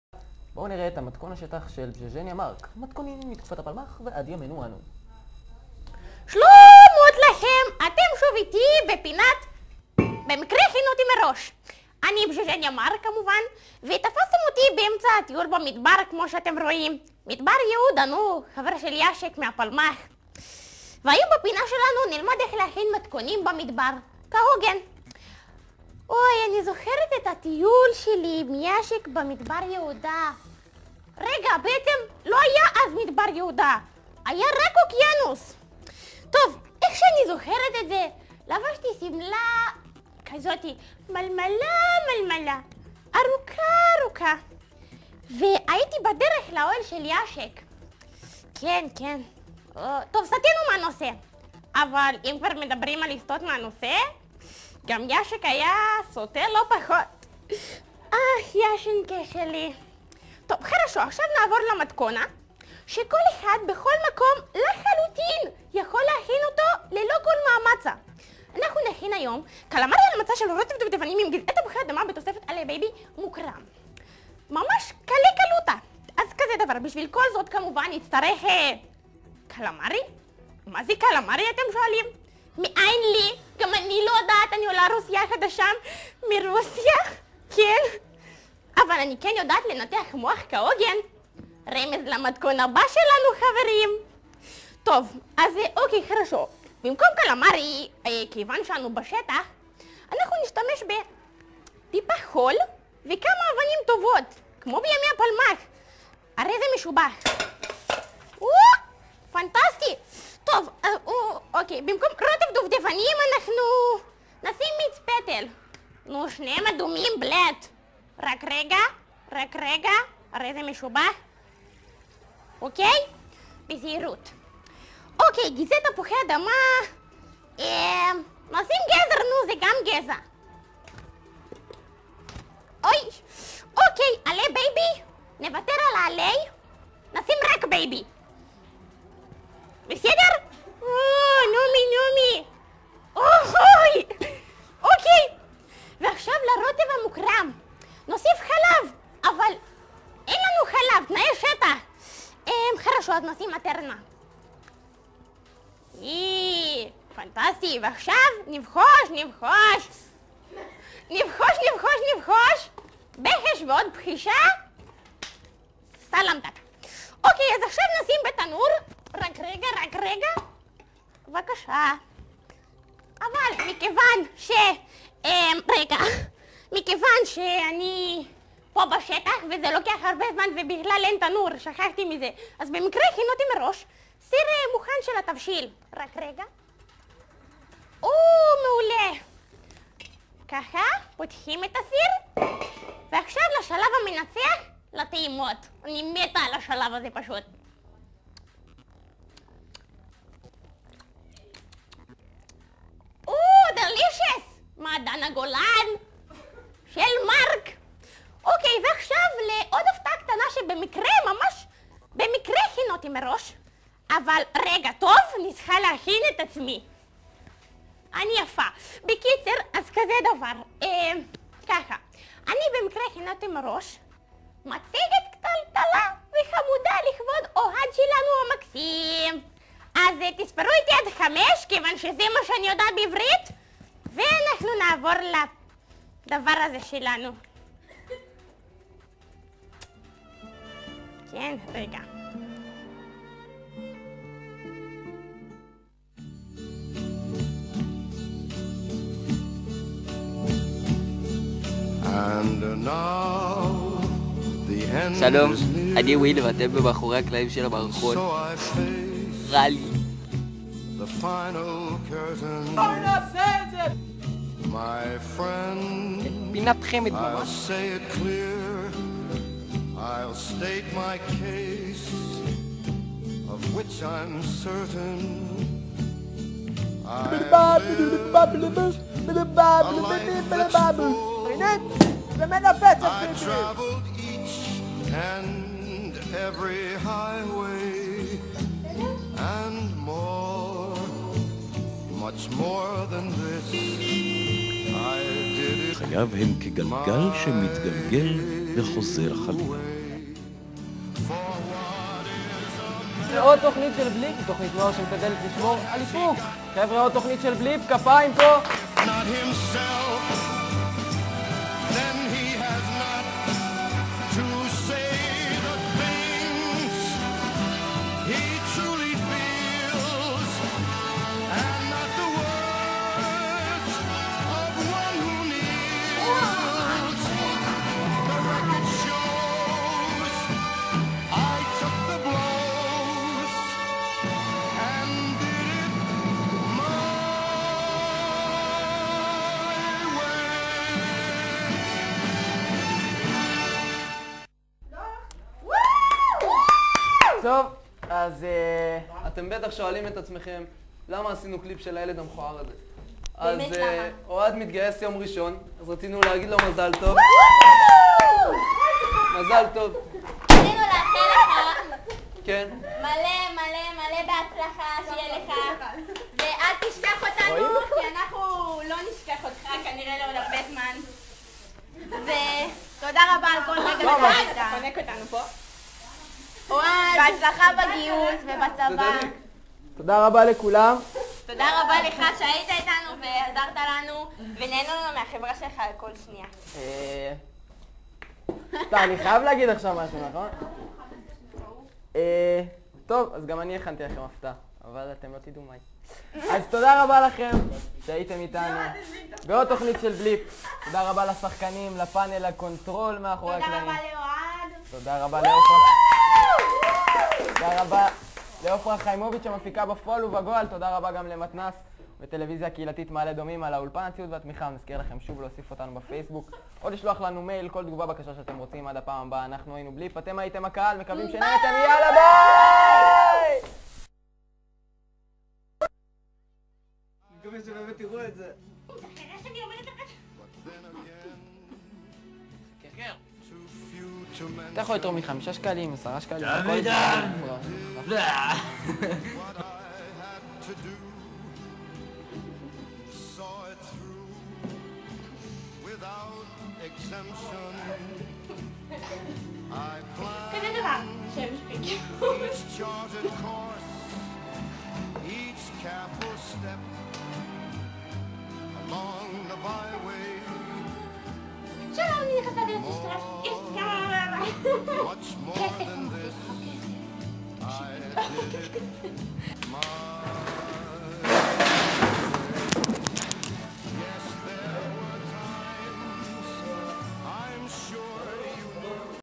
פינת הכינותי מראש עם בג׳ג׳ניה - מתכון במדבר
הפינה של בג'גניה שהפעם עם מתכון מיוחד לקלמרי ברוטב דובדבנים וגם בפרידה מרגשת מאוהד שמתגייס לצה"ל